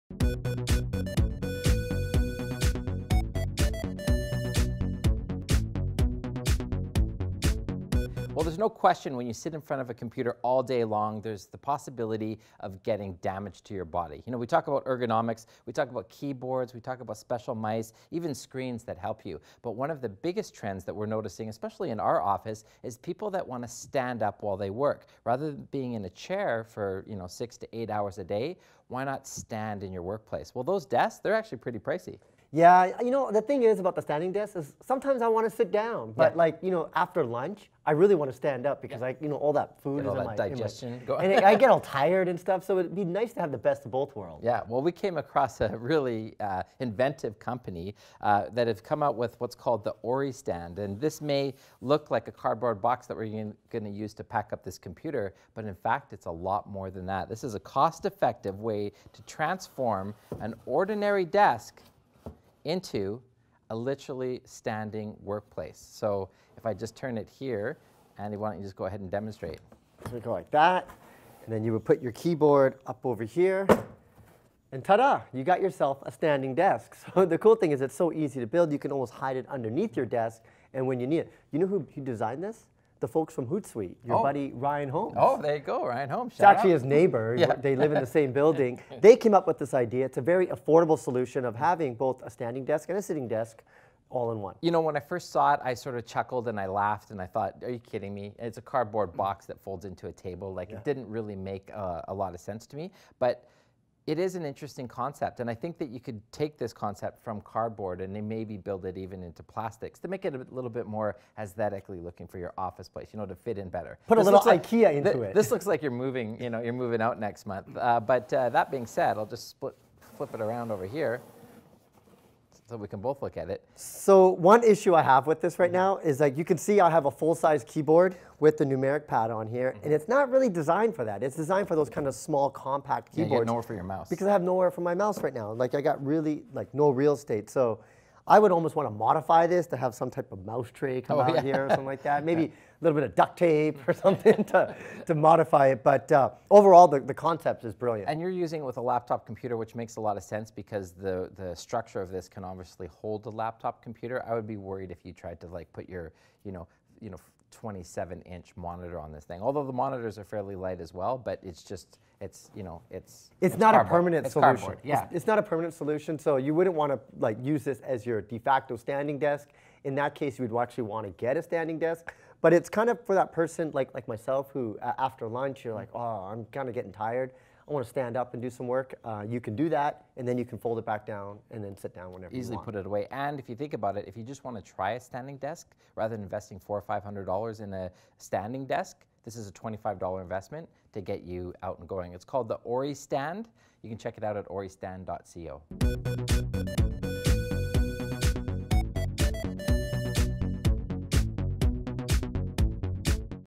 Affordable and Eco-Friendly Standing Desk | GetConnected
Back problems at the office bringing you down? The Oristand is an inexpensive, portable standing desk that can be installed and put away within seconds.